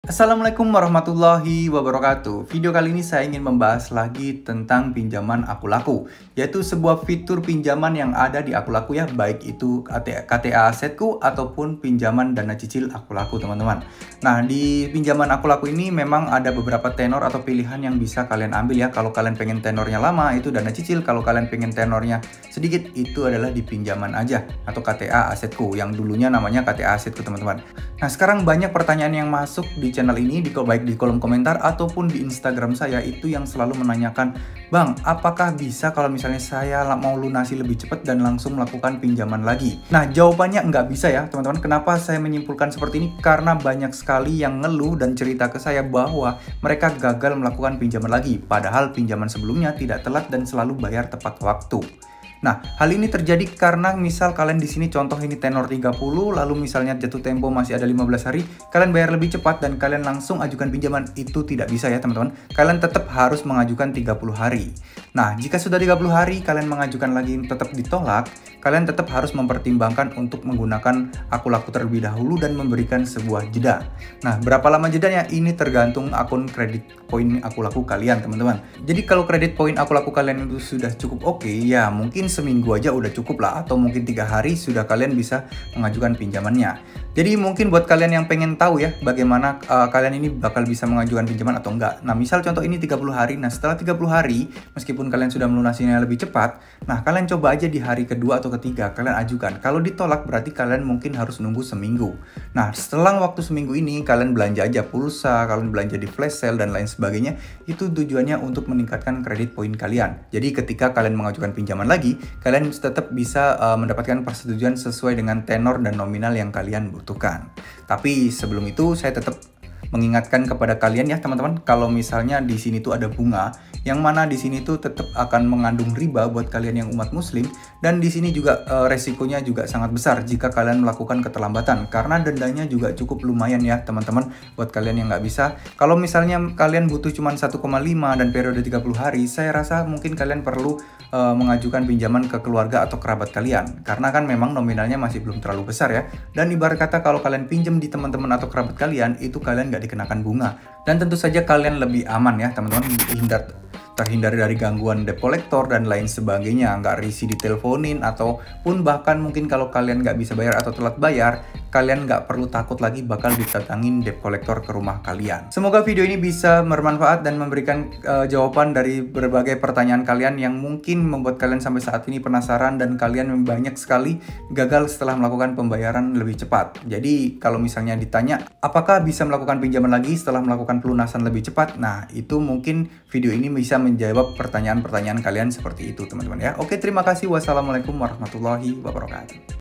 SETELAH MELUNASI AKULAKU LEBIH CEPAT APAKAH BISA LANGSUNG PINJAM LAGI ? PINJAMAN ONLINE AKULAKU !
Video kali ini saya akan membahas tentang pelunasan tagihan pinjaman online akulaku. Banyak dari teman-teman disini yang ingin bertanya apakah bisa jika mereka melakukan pelunasan tagihan akulaku lebih cepat, lalu kemudian mereka langsung mengajukan pinjaman lagi?

Untuk mengajukan pinjaman akulaku lagi, menurut saya tidak bisa secara langsung setelah pelunasan. Banyak pengalaman temen-temen disini bercerita kalau pinjaman akulaku mereka ditolak ketika mereka mengajukan ke dua kalinya setelah melakukan pelunasan pinjaman akulaku sebelumnya. Jadi bisa diambil kesimpulan bahwa melakukan penulasan pinjaman akulaku lebih cepat belum tentu bisa mengajukan pinjaman lagi.